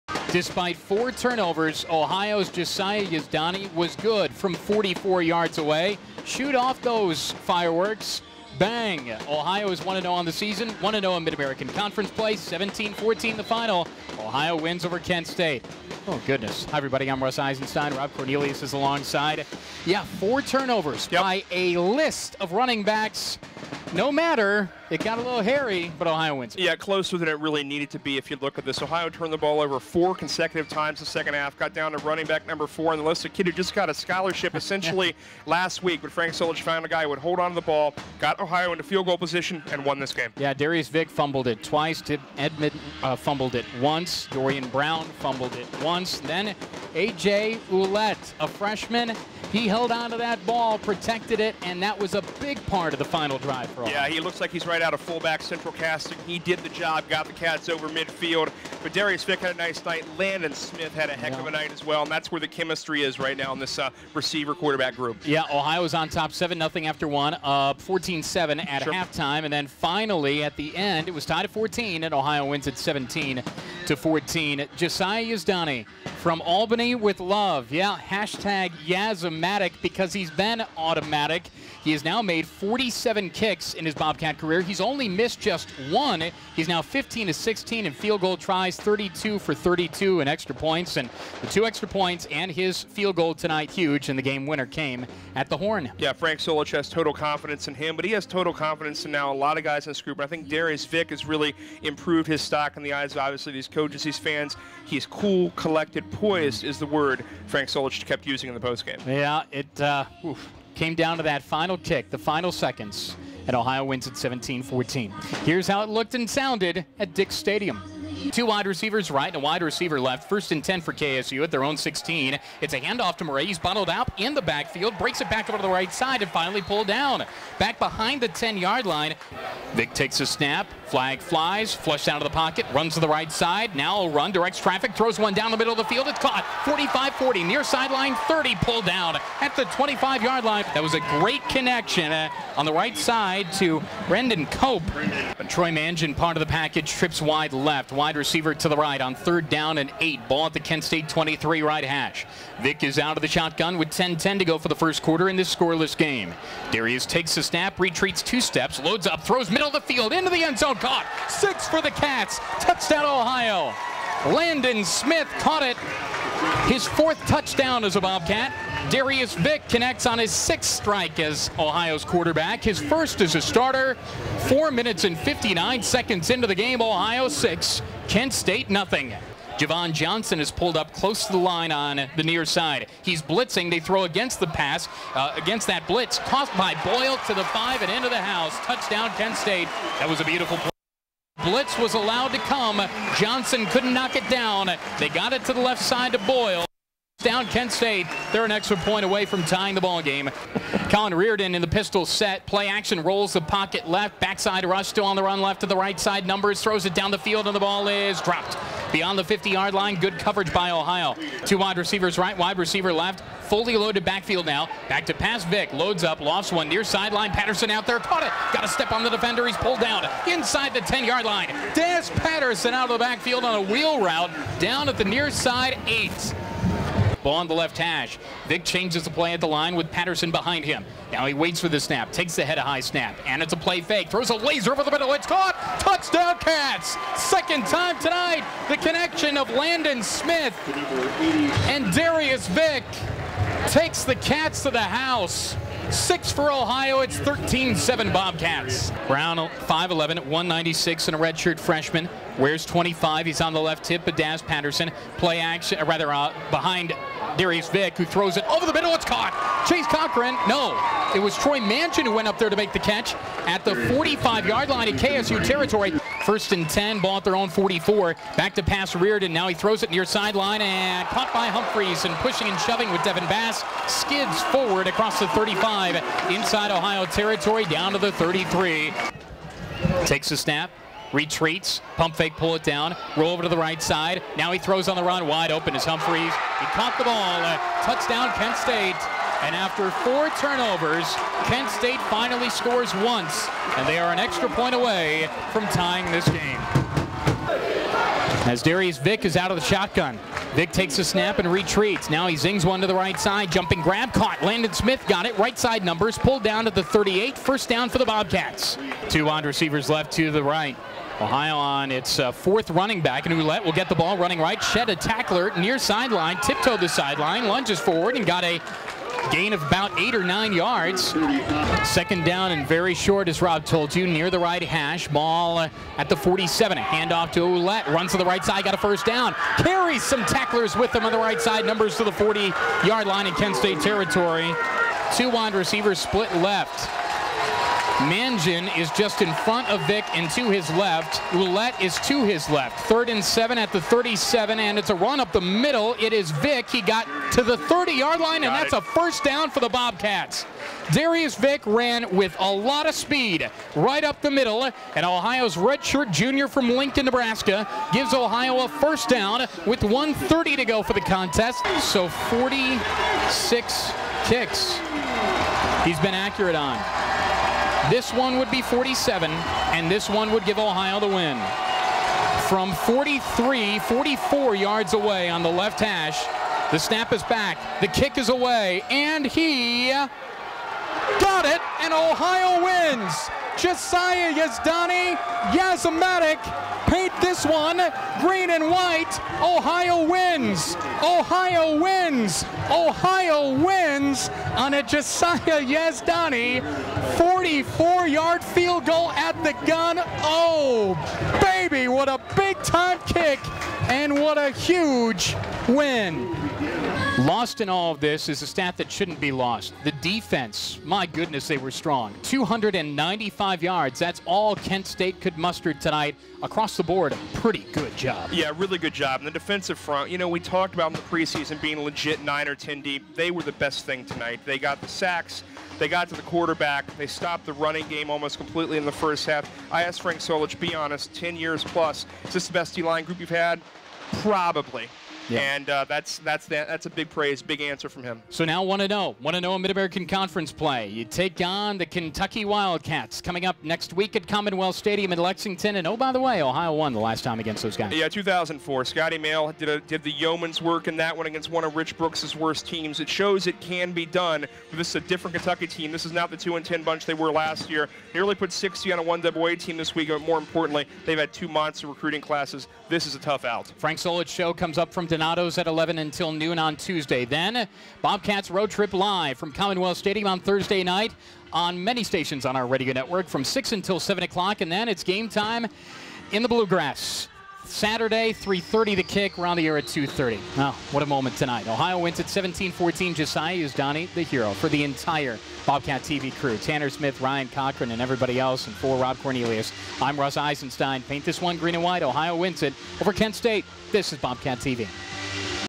Ohio Football 2014: Kent State Recap (August 30, 2014)
The Bobcat Radio crew recap Ohio's thrilling 17-14 win on a last second 44-yard field goal by kicker Josiah Yazdani.